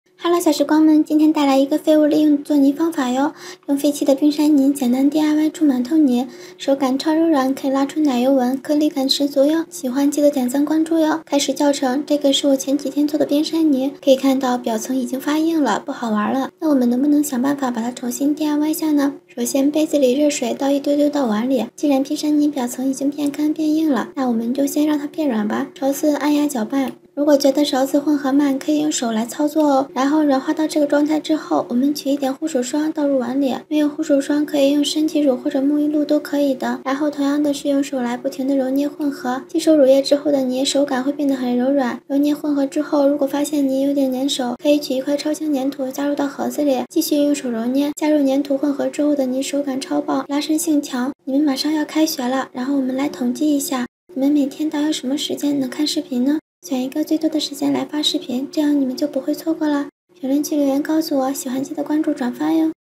无胶水无硼砂DIY馒头泥，只需废弃的冰山泥，手感细腻颗粒感十足
用废弃的冰山泥DIY馒头泥，手感超棒颗粒感十足，无硼砂 "请放心，这个视频专注于美食的制作过程，所有食材的获得方式都符合道德和法律规定。"